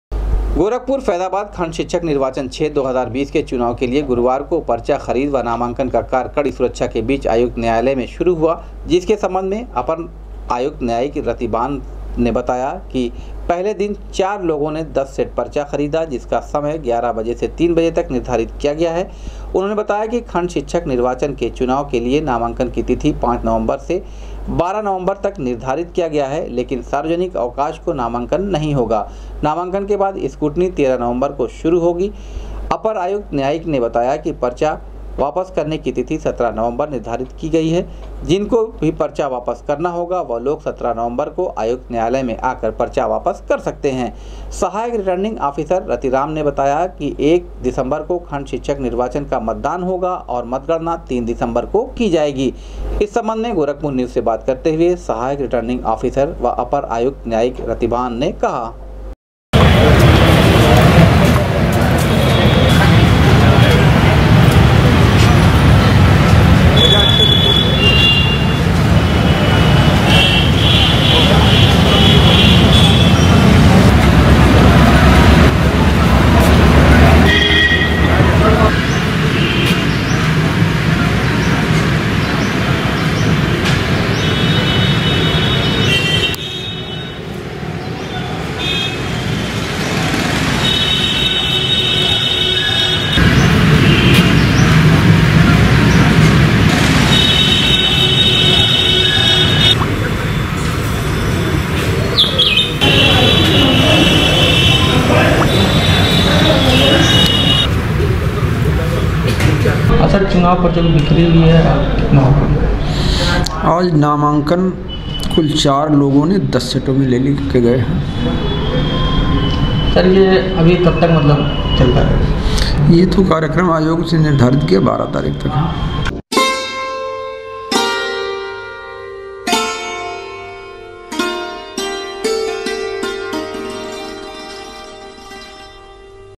गोरखपुर- फैजाबाद खंड शिक्षक निर्वाचन क्षेत्र के चुनाव के लिए गुरुवार को शुरू हुई नामांकन प्रक्रिया
गोरखपुर- फैजाबाद खंड शिक्षक निर्वाचन क्षेत्र के चुनाव के लिए गुरुवार को शुरू हुई नामांकन प्रक्रिया, चार प्रत्याशियो ने खरीदा पर्चा,एक दिसम्बर को मतदान व तीन को होगी मतगणना